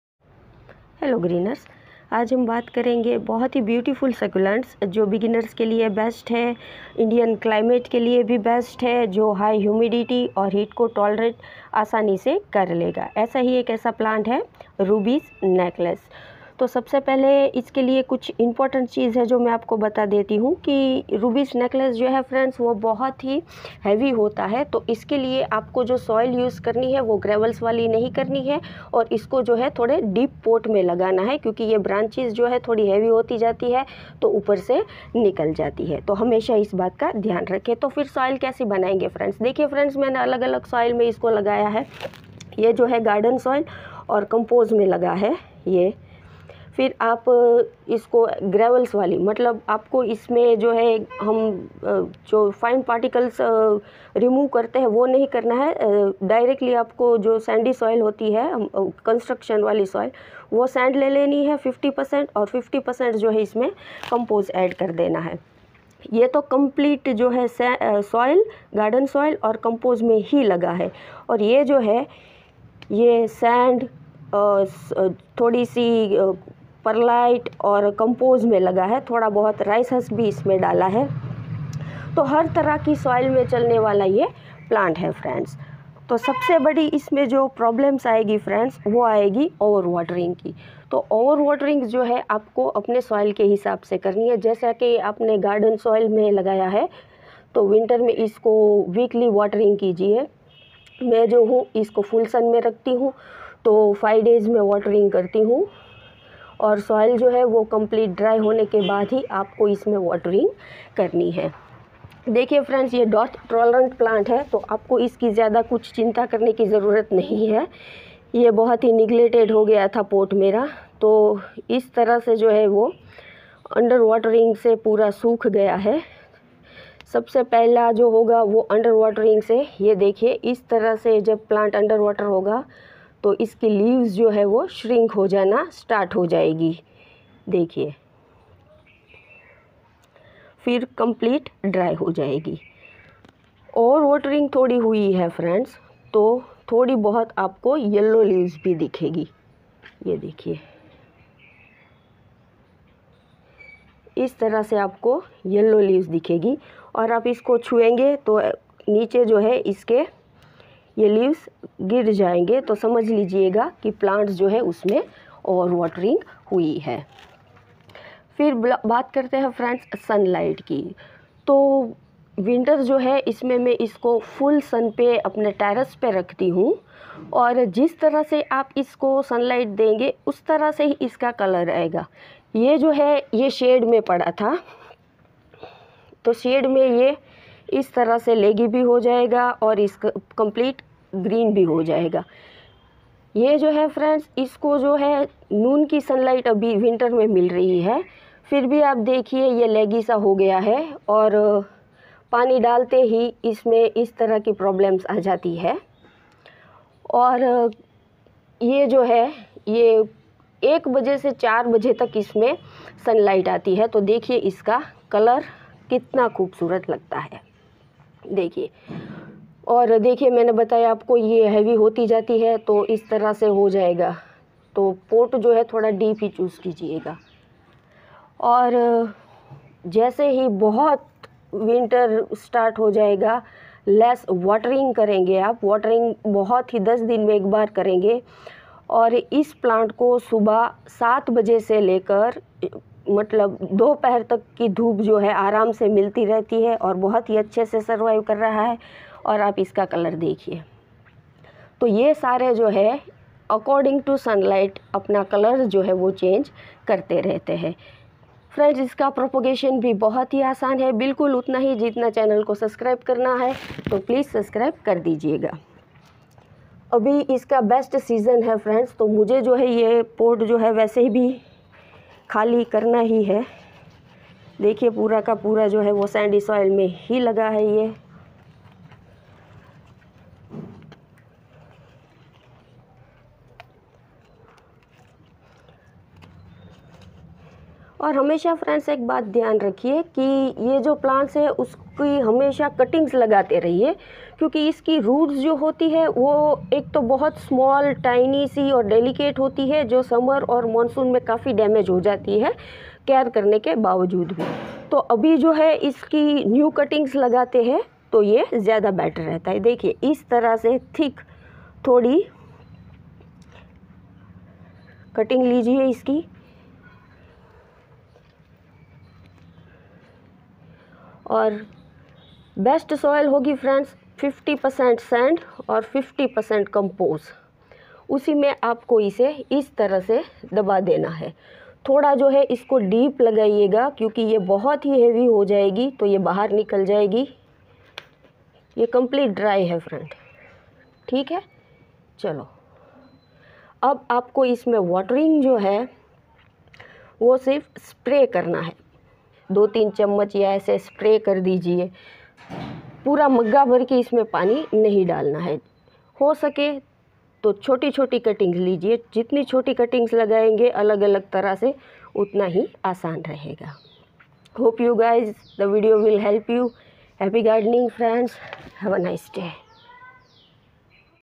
how to care for Ruby's necklace succulent/little pickles succulent/soil,light nd propagation
how to care for Ruby's necklace succulent plant

othonna capensis succulent care

Ruby's necklace succulent care

little pickles succulent plant

in this video i give full information about Ruby's necklace regarding indian climate

topic covered
1 water nd soil for succulents plants
2 pot information
3 propagation nd light requirements
4 reason of different colours for plants
5 growing conditions of this plants